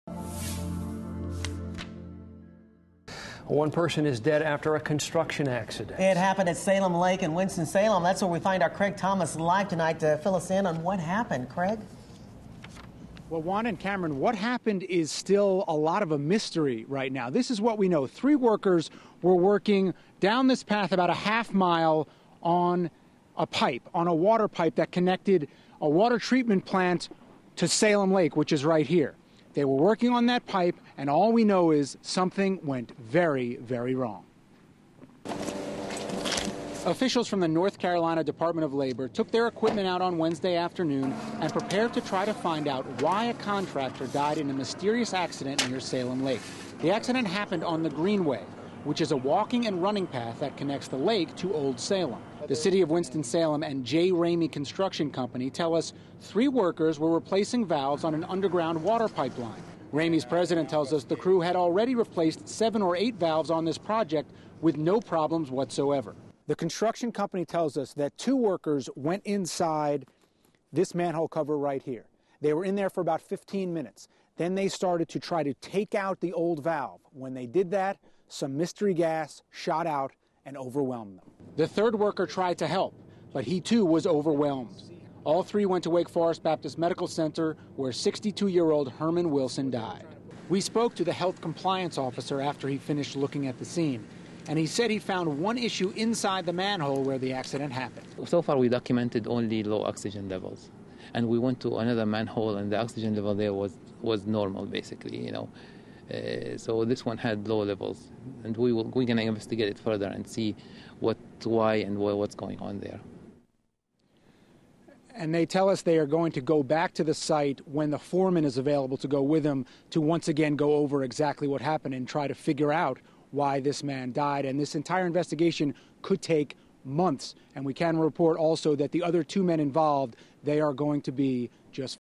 Contractor Killed During City Repair Project
Herman Wilson, 62, died while replacing a water pipe in Winston-Salem.